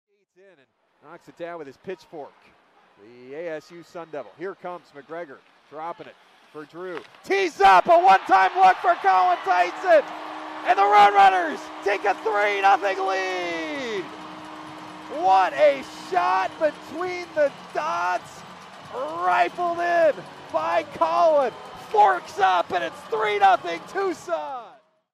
Arizona Theisen
Whether he is scoring goals at Arizona State in Tempe, or for the Tucson Roadrunners at the Tucson Arena, Colin Theisen has to enjoy playing hockey in the state of Arizona. Theisen, recently recalled from Toledo, scored his second goal of the year for the Roadrunners in the second period of Saturday night's contest. Theisen was assisted by former collegiate teammate and Roadrunners leading scorer Josh Doan.